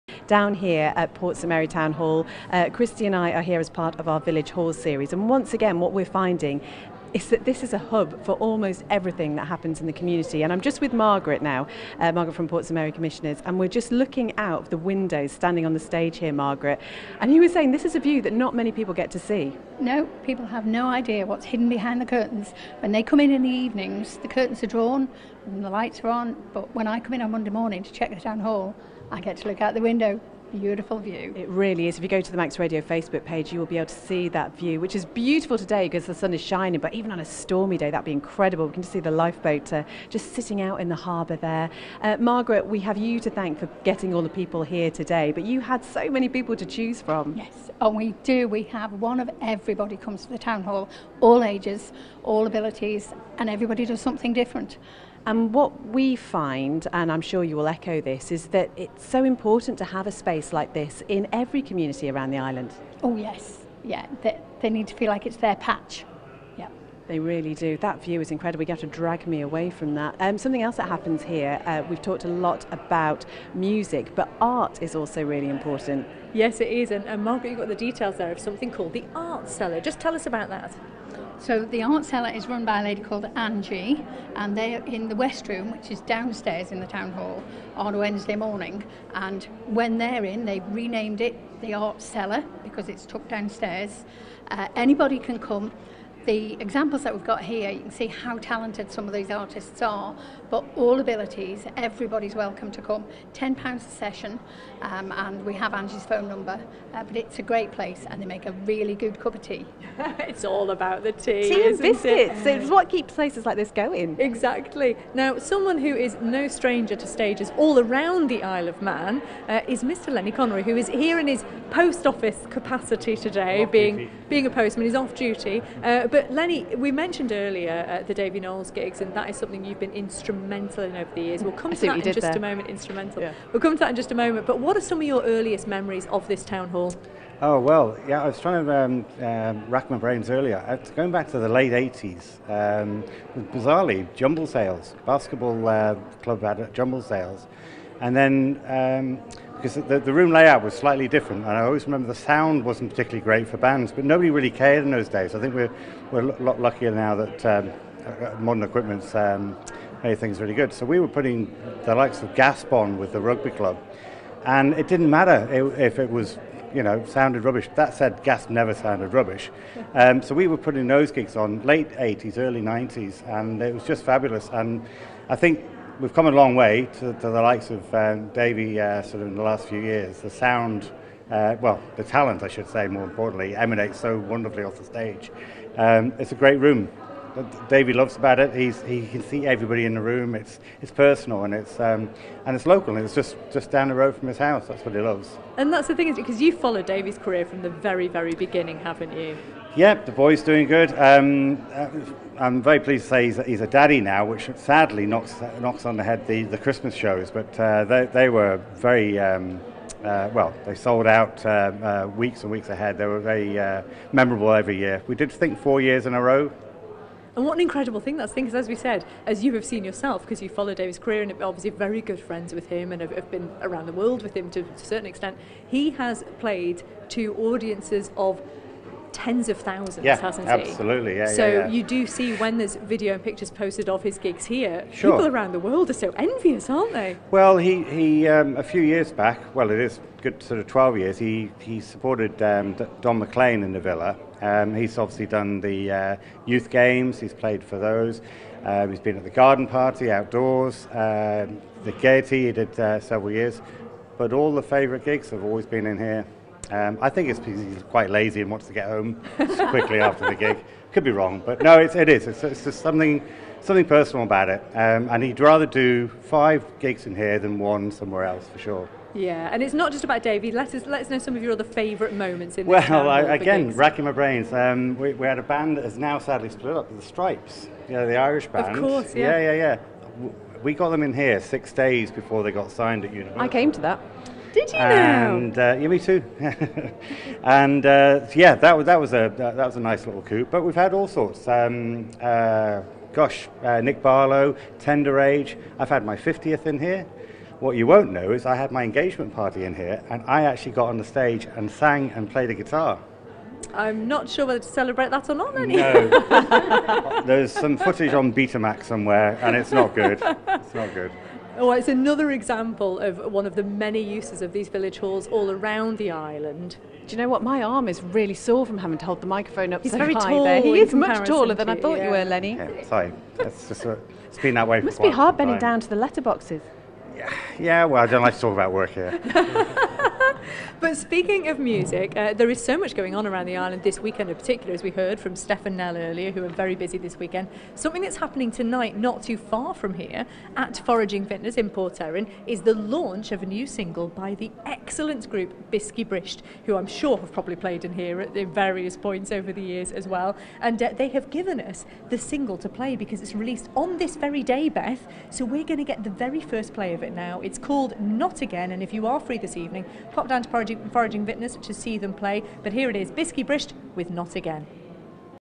Village Hall Series live in Port St Mary - Part Two
In the latest edition of our VillageHallSeries Beth and Christy stop off at PSM Town Hall. Here they find out about some of the artistic uses for the hall and some legendary live music performances.